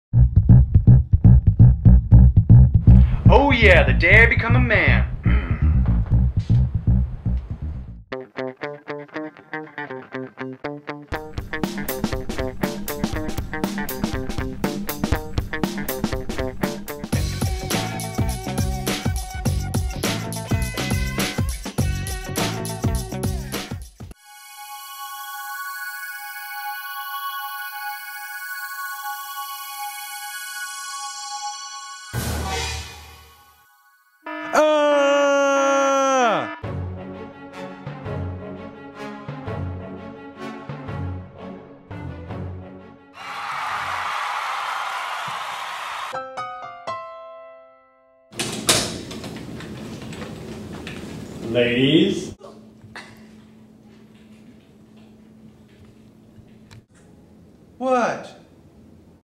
Gillette Parody
This commercial parody of Gillette was made in the spring of 2011.   It was for the ASM 4M1 class.